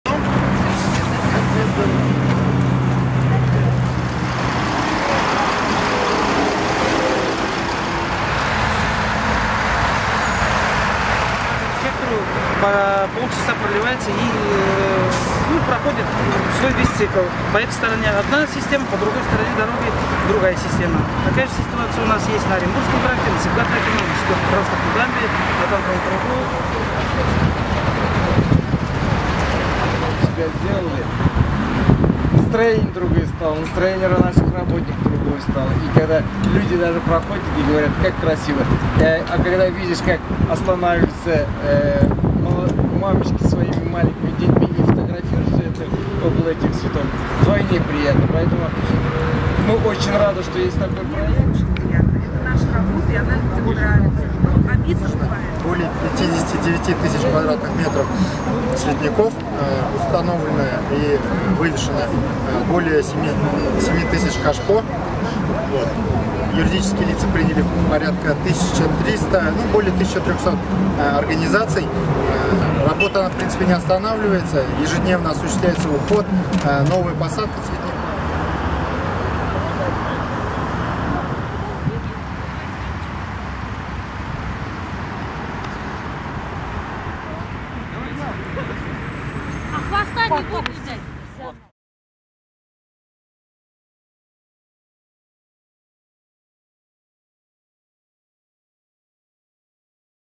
На жар-птицу у стен Казанского кремля ушло 36,5 тыс. цветов
В Казани в этом году цветники разбиты на площади 59 тыс. кв. м, установлено 7,7 тыс. кашпо и вазонов. Сегодня журналистам в ходе пресс-тура рассказали, как реализуется проект «Цветущая Казань» и как «Горводзеленхоз» ухаживает за зелеными насаждениями.

Съемка и монтаж: Сергей Елагин